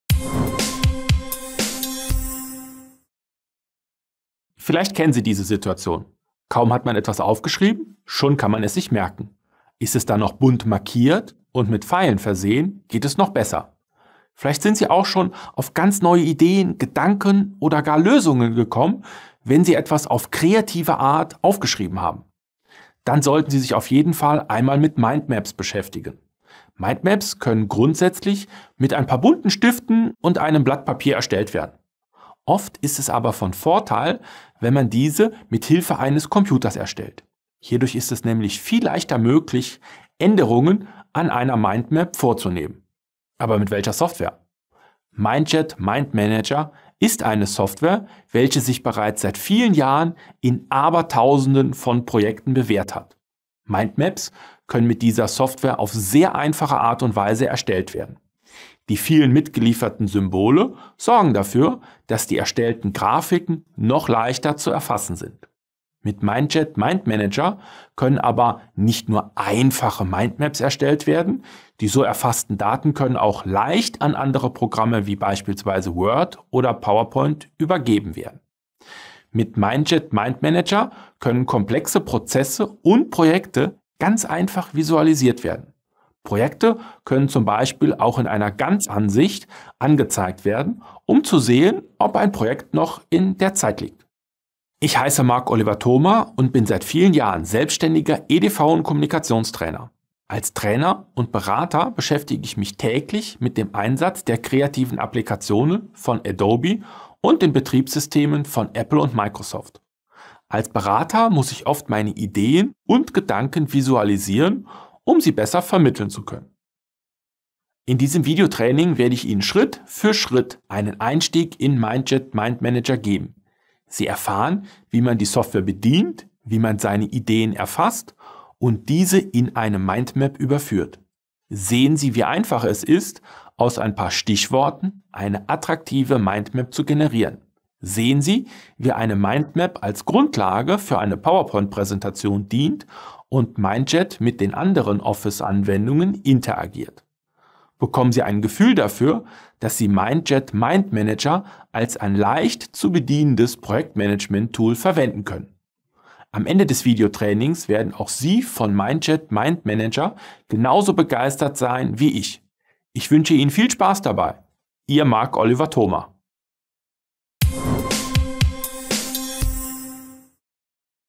MindManager – Grundlagen Tutorial: Trailer |video2brain.com
Komplettes Video-Training MindManager von Mindjet ist das bekannteste Werkzeug für die Erstellung von Mindmaps. Marc Oliver Thoma zeigt Ihnen in diesem Video-Training, was es mit Mindmaps auf sich hat und wie Sie damit Gedanken, Projekte und Prozesse visualisieren. Schritt für Schritt lernen Sie, wie Sie MindManager bedienen, Ihre Ideen erfassen und diese in eine attraktive Mindmap überführen. Sie sehen auch, wie eine Mindmap mit Microsoft Office-Anwendungen interagiert und als Projektmanagement-Tool dienen kann.
Dieses Tutorial ist ein Auszug aus Marc Oliver Thomas neuem Video-Training „MindManager – Grundlagen. Mindmaps erstellen und weiterverarbeiten.“